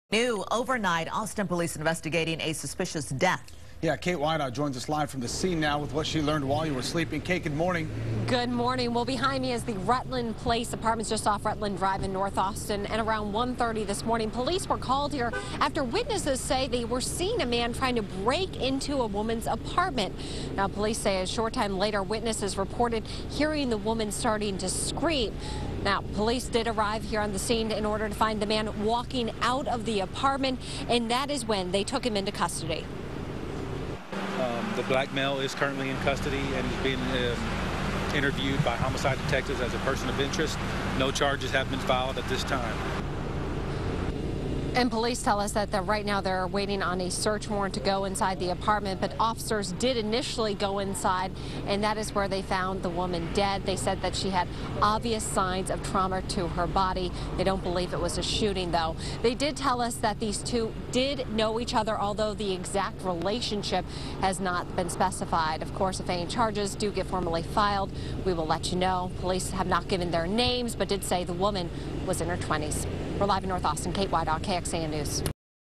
Overnight homicide
Austin police are investigating a suspicious death that happened overnight.